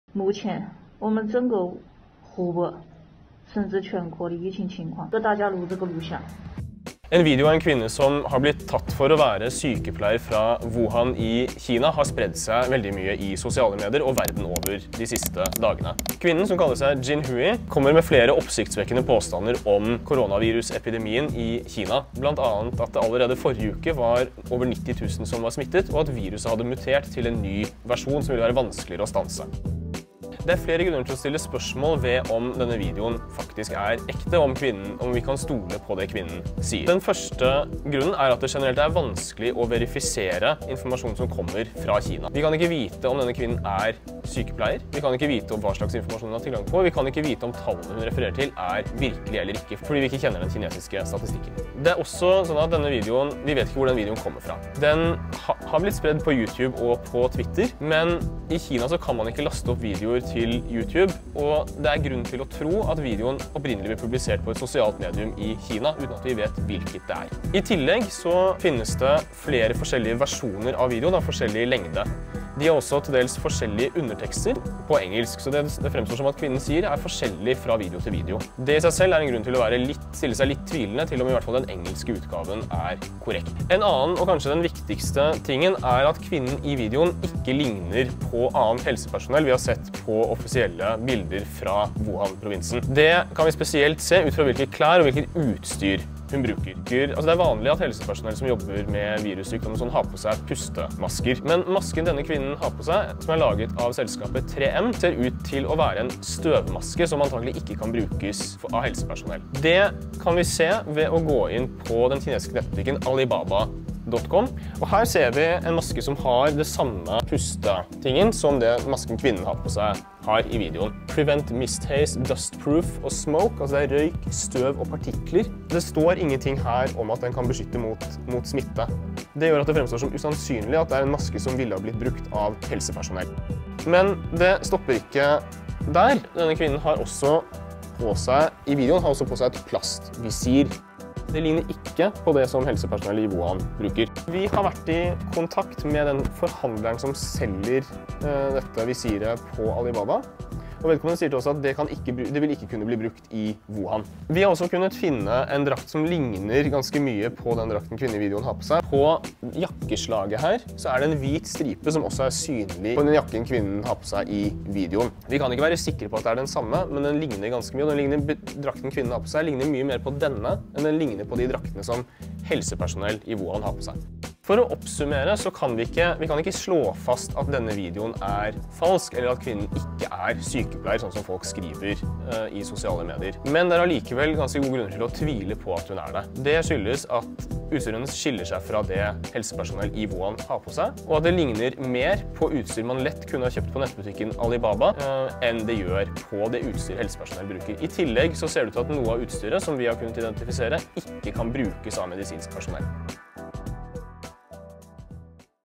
Hevder 90 000 er smittet av koronavirus. Kan du stole på det?
En video av en kvinne som påstås å være sykepleier i Wuhan-området i Kina har den siste uken blitt spredd i sosiale medier. Kvinnen hevder at over 90 000 personer skal være smittet av koronaviruset. I Norge har blant andre Nettavisen og Resett publisert videoen.

Vi har forsøkt å ettergå videoen - dette har vi funnet ut: